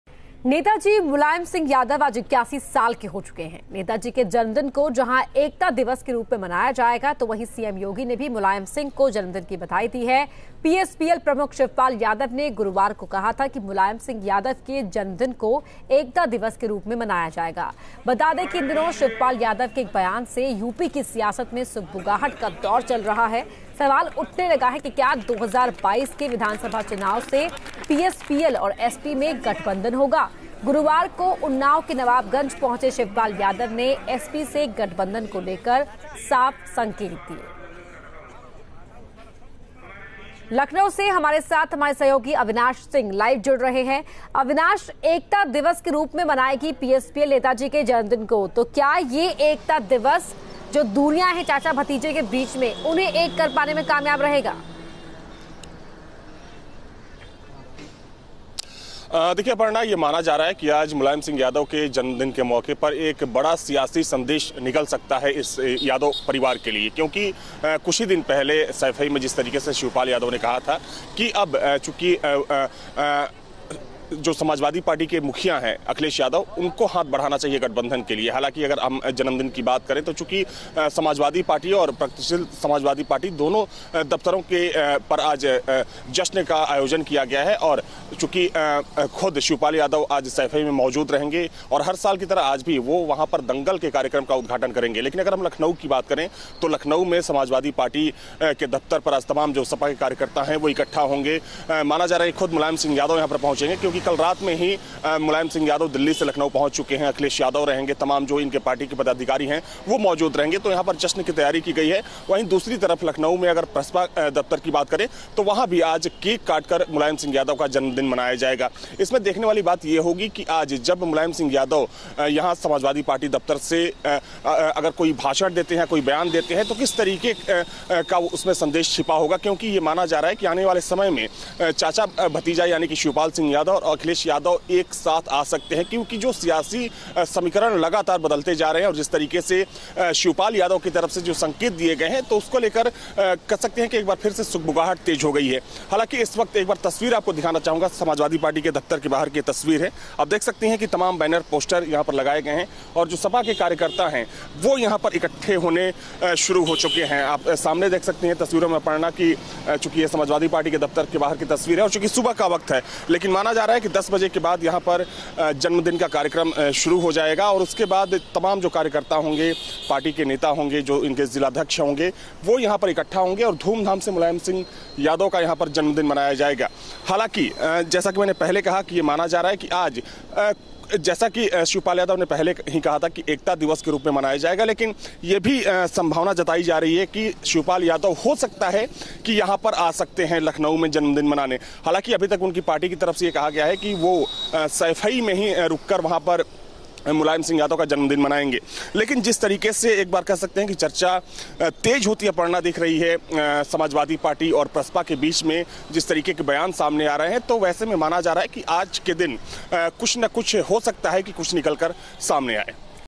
Uttar pradesh: मुलायम सिंह के जन्मदिन को एकता दिवस के रूप में मनाएंगे शिवपाल यादव
समाजवादी पार्टी (Samajwadi Party) के संरक्षक मुलायम सिंह यादव (Mulayam Singh Yadav) आज अपना 81वां जन्मदिन मना रहे हैं. उनके जन्मदिन को लेकर समाजवादी पार्टी (Samajwadi Party) के कार्यकर्ता काफी उत्साहित हैं. इस मौके पर जिला स्तर पर कई कार्यक्रम आयोजित किए जा रहे हैं. मुलायम सिंह यादव (Mulayam Singh Yadav) के छोटे भाई व प्रसपा अध्यक्ष शिवपाल सिंह यादव (Shivpal Singh Yadav) भी इस मौके को खास बनाने के लिए बड़ा कार्यक्रम कर रहे हैं.

News State Apps डाउनलोड करें:
Download Android and iOS Apps for more updates: